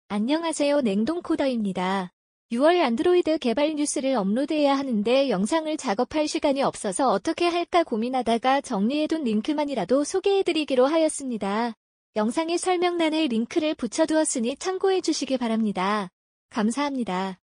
냉동코더의 안드로이드 개발 뉴스::23.06
2023년 6월 안드로이드 개발관련 뉴스입니다.

- How to architect Android apps: a deep dive into principles, not rules

- Why should you always test Compose performance in release?

- Learn about our updated renderer for the Maps SDK for Android